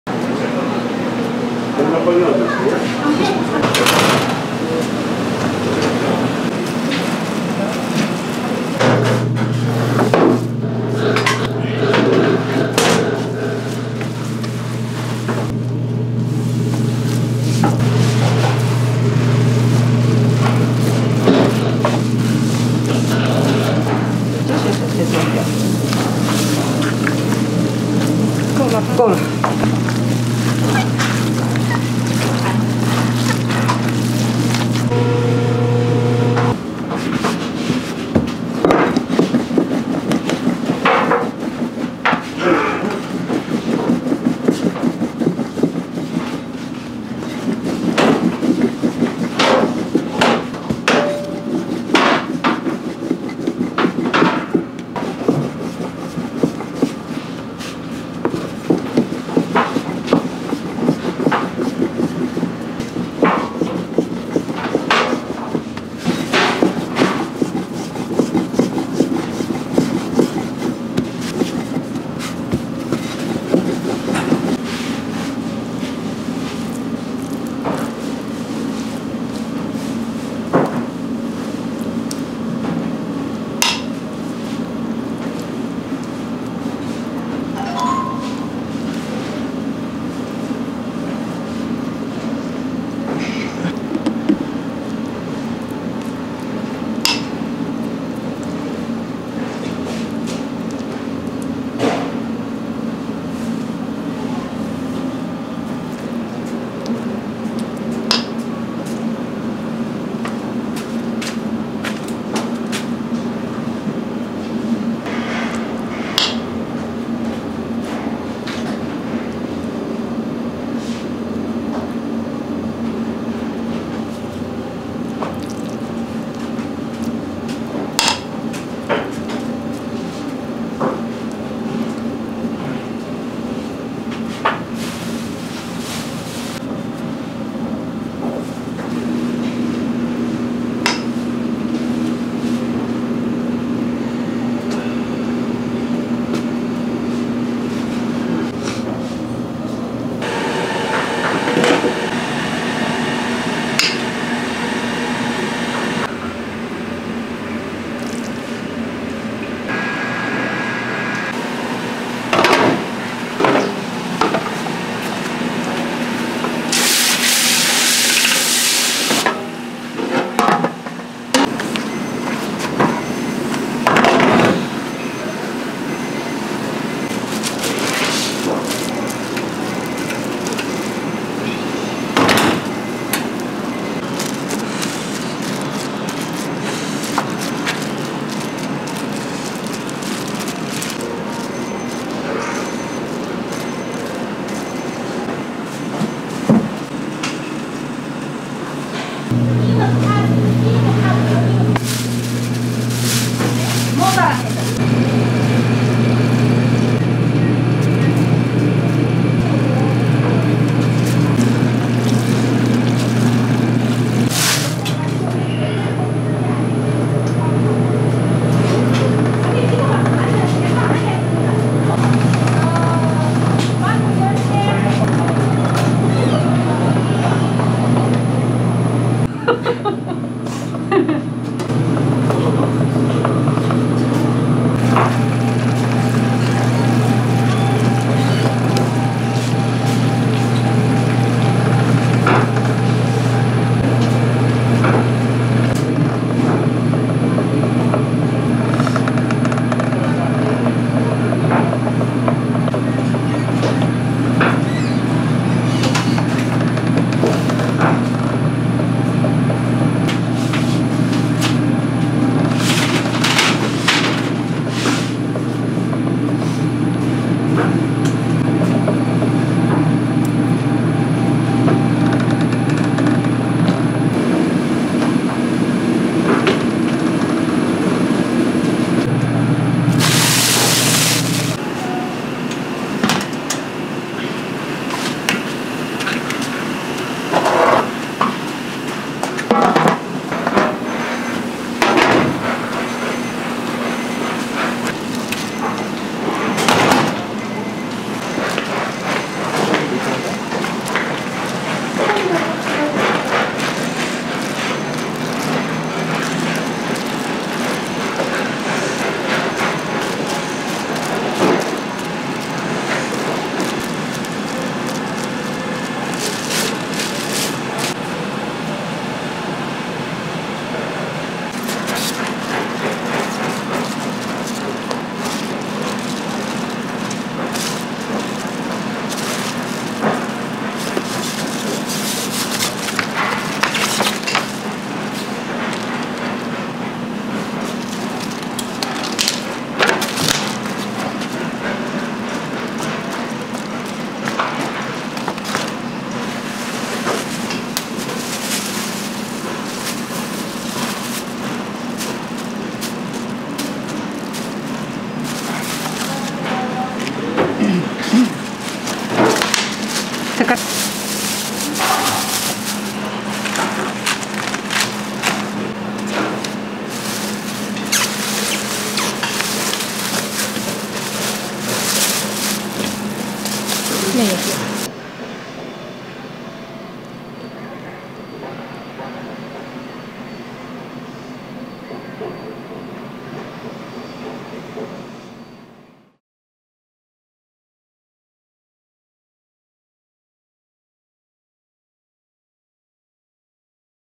신포시장 포동포동 고기만두, 김치만두┃Meat Dumplings & Kimchi Dumplings, Mandu┃Korean Street Food
[신포시장 포통포통 고기만두, 김치만두┃Meat Dumplings & Kimchi Dumplings, Mandu┃Korean Street Food]

📢 Information

🥇 신포시장 산동만두[Sinpo Market]

💰 Meat Mandu, Kimchi Mandu: KRW 5,000(4.42$)✔

💰 김치만두, 고기만두 1팩: KRW 5,000✔

: 반죽부터 고기, 김치까지 직접 만드는 신포시장 유명 만두집. 김치만두는 손으로 빚고 고기만두는 기계로 뽑아낸다.

: A famous dumpling restaurant in Sinpo Market that makes dough, meat, and kimchi. Make kimchi dumplings by hand and pull out meat dumplings with a machine.

📍Location: 인천 중구 우현로49번길 33

👍  'MAYO FOOD'